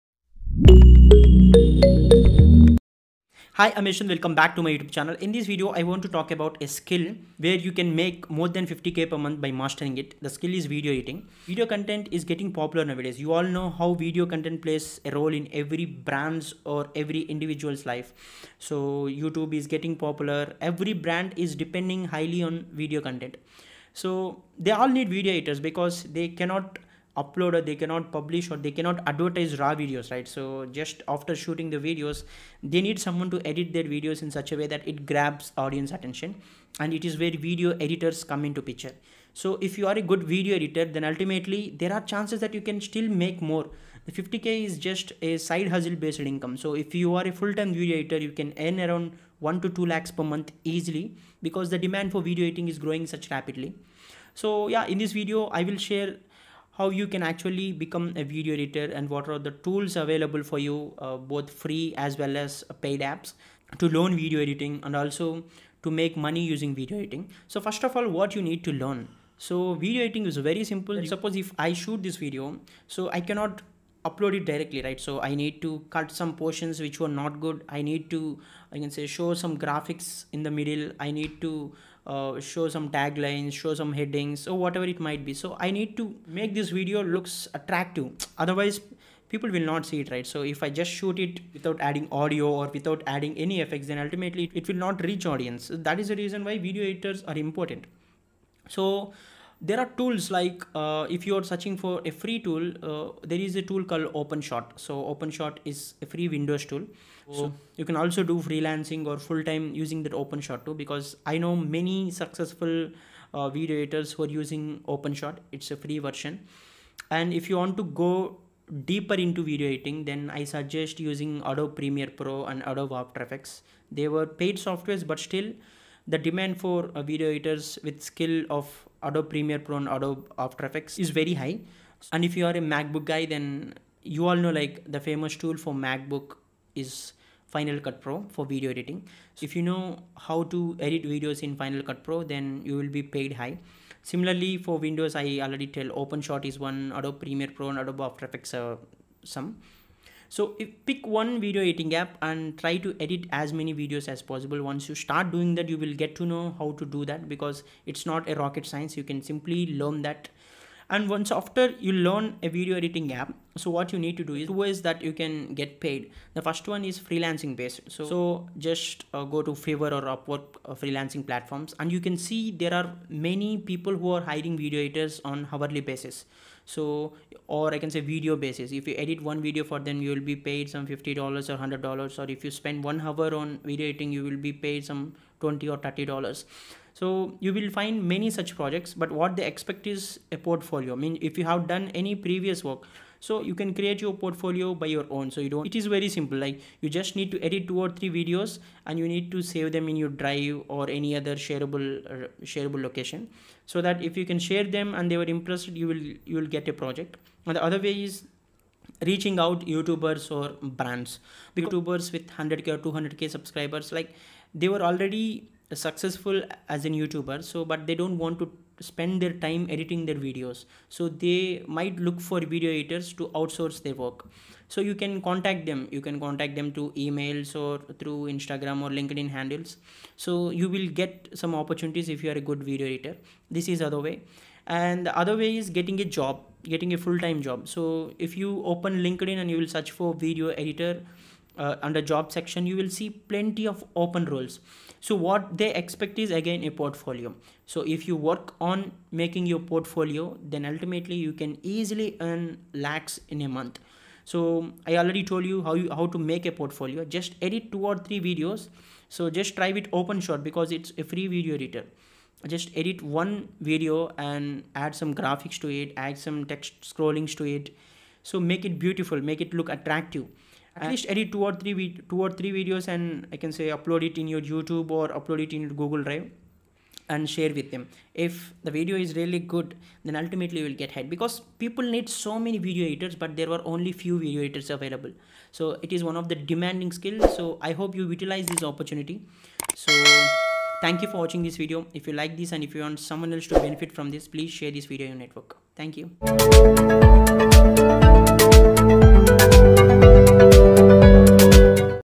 How To Earn More Than 50K Per Month By Editing Videos | Make Money As A Video Editor
Hello, all!

In this video, I've talked about ⬇️

What is Video Editing?
How do you get started as a Video Editor?
Tools/Softwares for Video Editing
How to earn through Video Editing?

Support me🤝❤️

Make money as a video editor
How to make money editing videos
Freelance video editor jobs